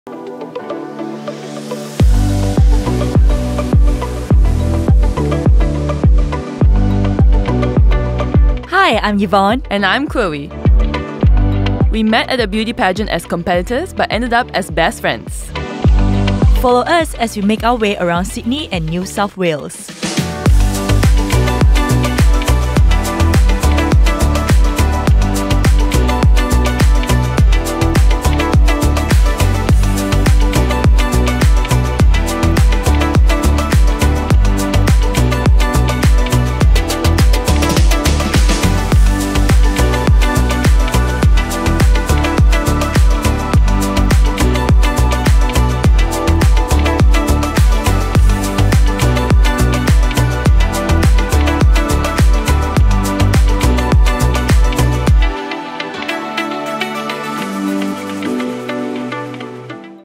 This is Sydney & New South Wales: Choose Your Path (Nature)
Best friends Yvonne and Chloe receive a letter inviting them on a journey of finding inspiration and new perspectives in Sydney and New South Wales. Join them as they make bold choices in discovering diverse experiences that will ignite their sense of adventure and fill their spirits with wonder.